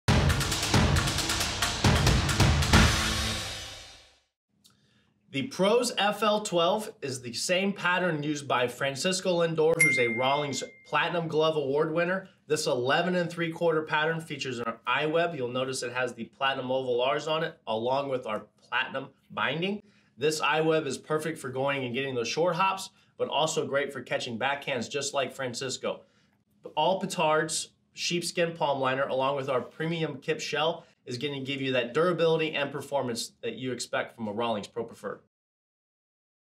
Rawlings U | 2021 Pro Preferred Francisco Lindor Glove
Learn more about the 2021 Pro Preferred Francisco Lindor glove here and see if it's right for you. This 11.75-inch classic beauty comes complete with a pro I-web and an unique, rare platinum "Oval-R" to commemorate his Platinum Glove Award win (2016).

Since this glove is also from our Pro Preferred series it was crafted with ultra-luxurious kip leather, and Pittards sheepskin lining for the best in performance and comfort. If you want to snag every bullet hit your way this year, you need to get your own PROSFL12B today!